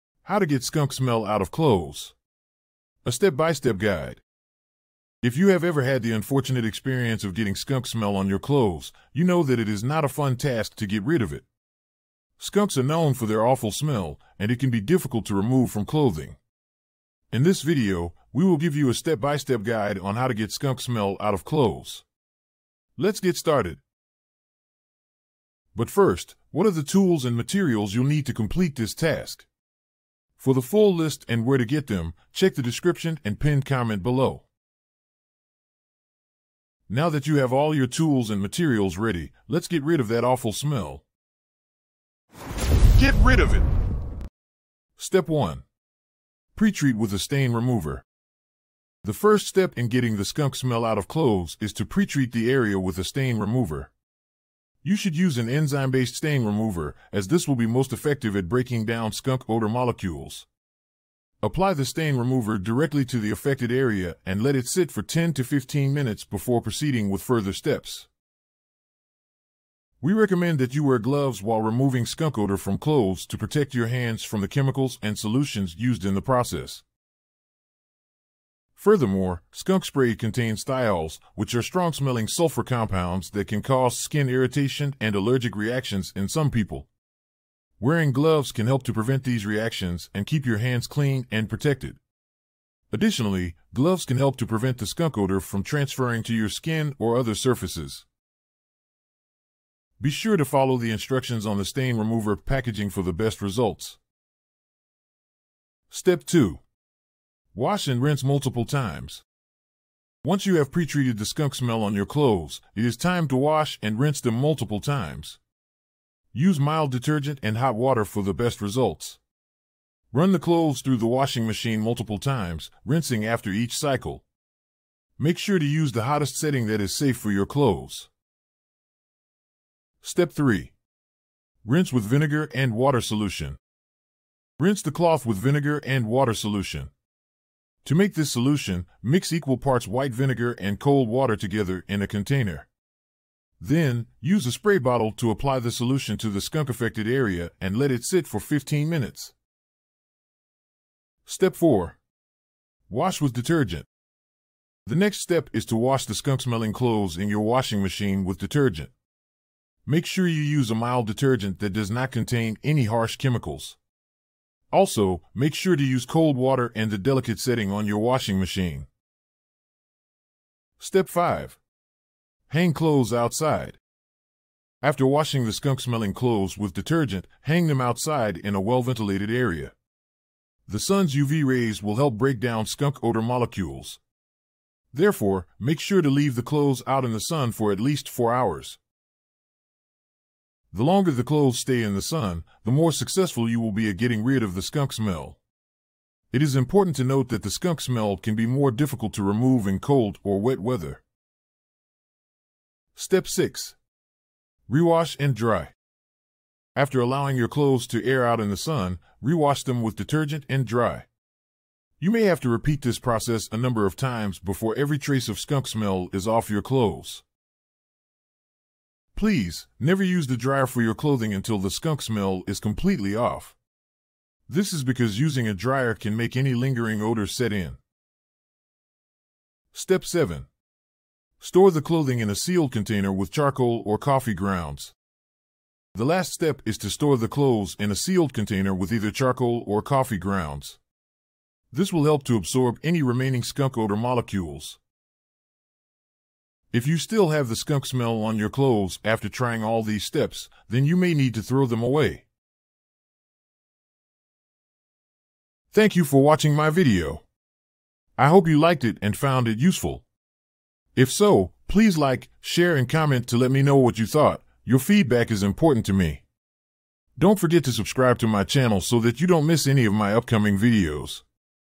How to Get Skunk Smell Out of Clothes [A Step-by-Step Guide]
Here's a short but detailed guide on how to get skunk smell out of clothes. Just follow the simple steps and your clothes will smell fresh again.
Things you need to complete this task...

Now that you have all your tools and materials ready, let's get rid of that awful smell...

Step 1: Pre-Treat with a Stain Remover
The first step in getting the skunk smell out of clothes is to pre-treat the area with a stain remover. You should use an enzyme-based stain remover, as this will be most effective at breaking down skunk odor molecules. Apply the stain remover directly to the affected area and let it sit for 10-15 minutes before proceeding with further steps.

We recommend that you wear gloves while removing skunk odor from clothes to protect your hands from the chemicals and solutions used in the process.